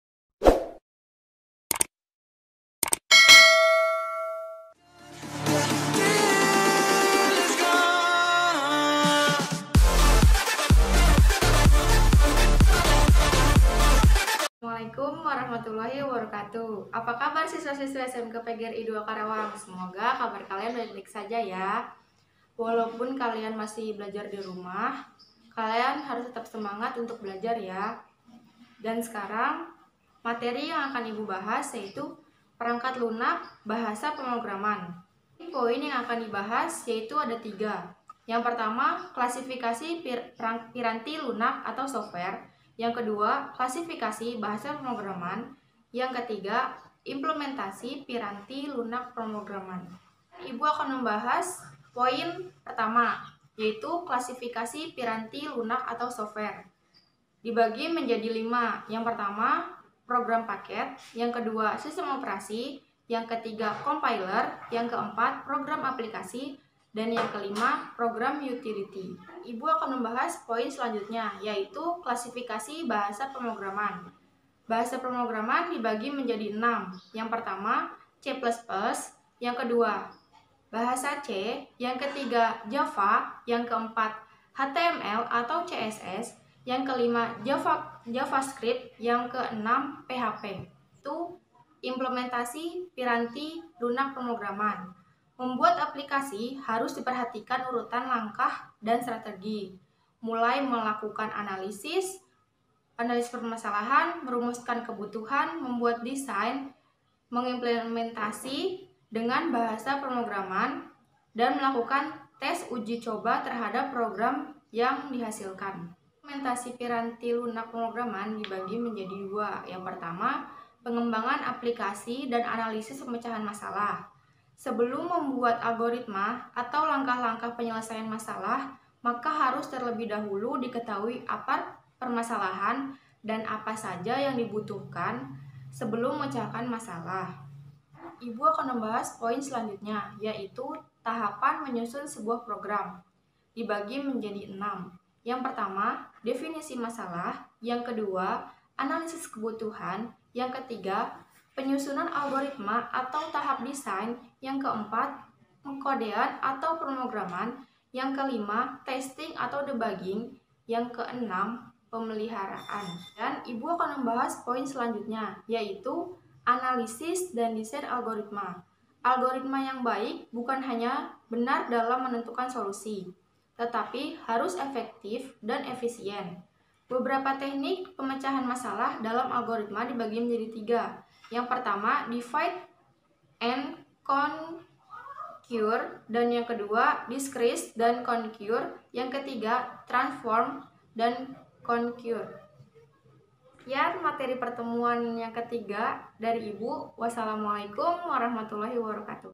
X_Pemrograman Dasar_Pertemuan ke-3
Pembelajaran Jarak Jauh Mata Pelajaran PEMROGRAMAN DASAR kelas X pertemuan ke-3

Dalam Pandemi ini SMK PGRI 2 KARAWANG, membuat salah satu Metode Pembelajaran Jarak Jauh yaitu dengan Video Pembelajaran yang memanfaatkan Youtube.